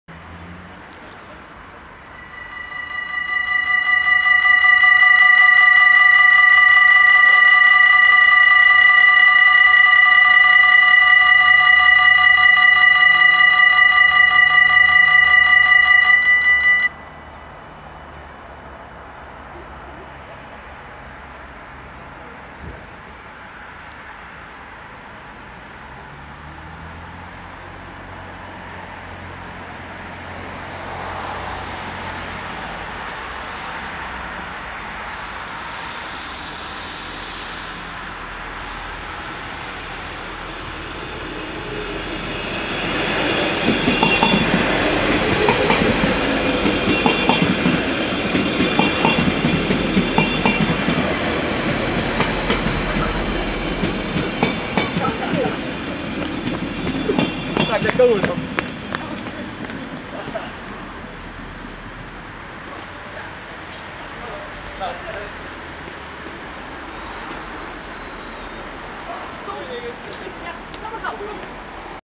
Spoorwegovergang Zevenaar // Dutch railroad crossing
Locatie: Station Zevenaar
Traject: Spoor 1 + 2
Soort: AOB
Bellen: 2
Bomen: 2
Passeren:
- GTW-D als ST Winterswijk → Arnhem